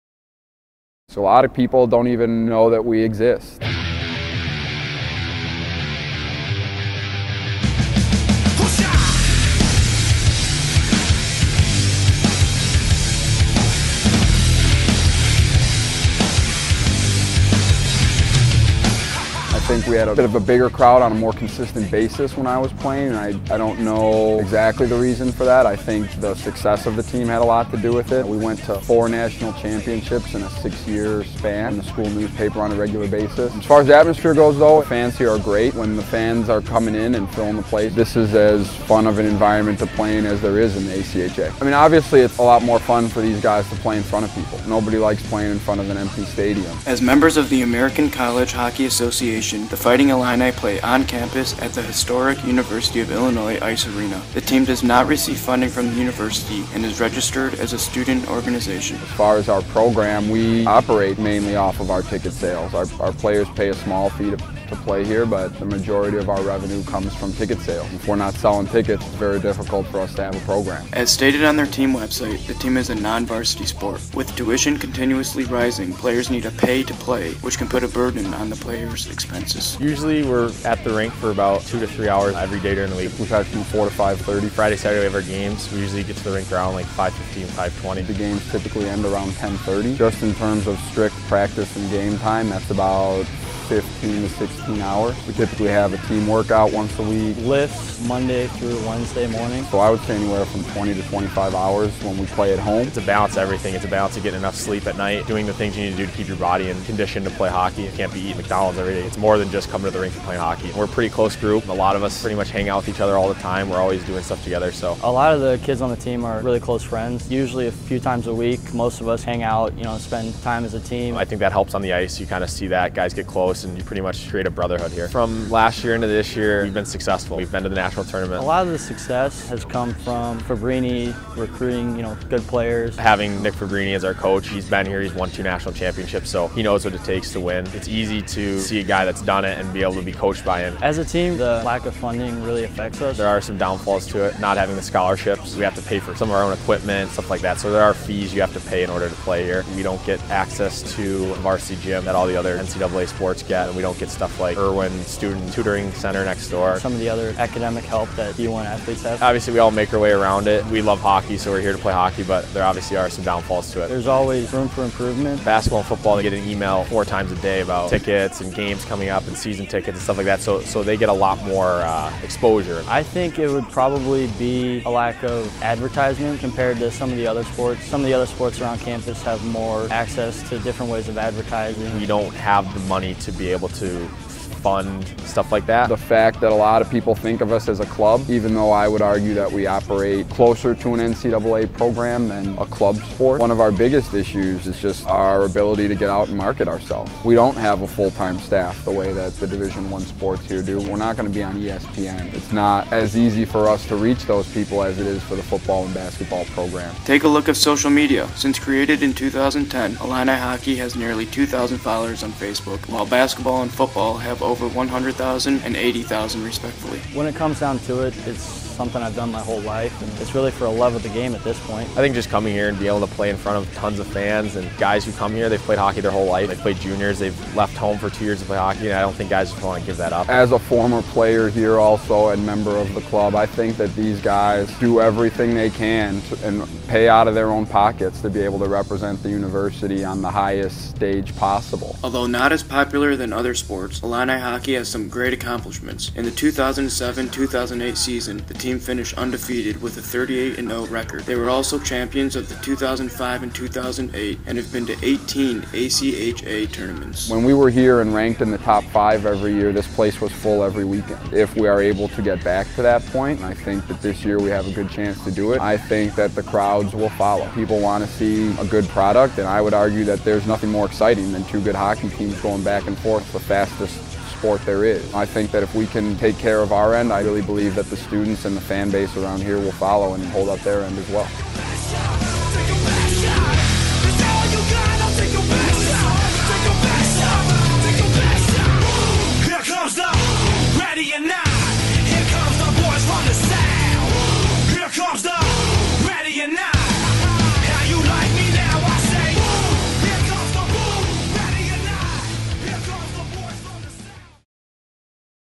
Illini Hockey - 2015
This is a video depicting the Illini Hockey Club at the University of Illinois and their presence on campus!